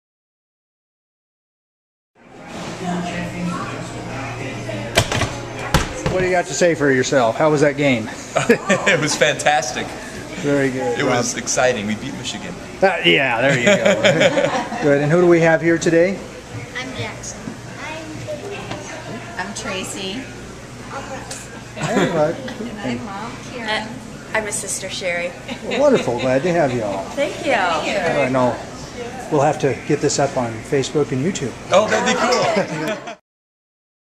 ASU Football 1987 Rose Bowl Player Rob Lessig and Family
We were happy today to find former ASU player Rob Lessig pointing himself out to his children  in the 1987 Rose Bowl team photo in the bar at Monti's!